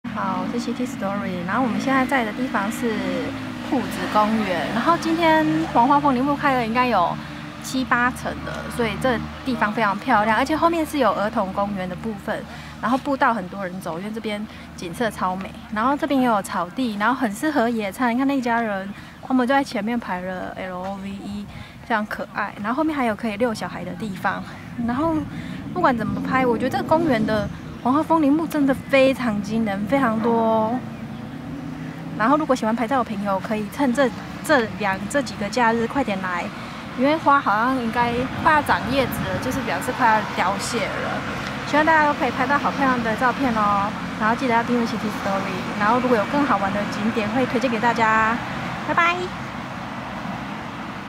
【臺中市北屯區景點】廍子公園-台中最美黃金公園怎能錯過賞花點!
如果說最美公路再芬園台14線，那你不能錯過最美的台中廍子公園，腹地廣且三月份花開時期最適合慢步